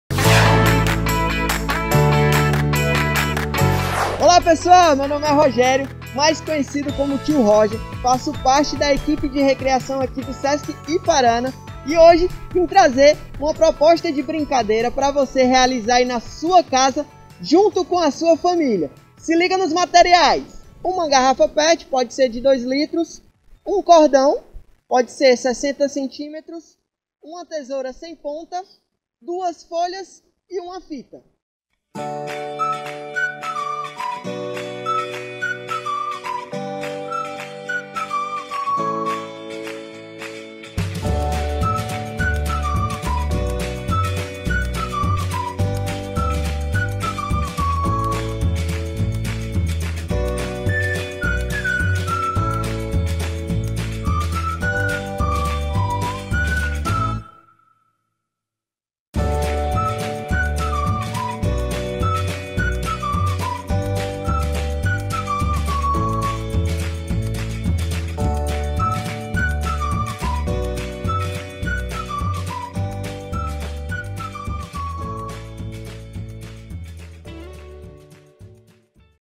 Bilboquê - DiaMundialDoLazerCeara
Aprenda a fazer um divertido Bilboquê para brincar em casa com toda a família no Dia Mundial do Lazer!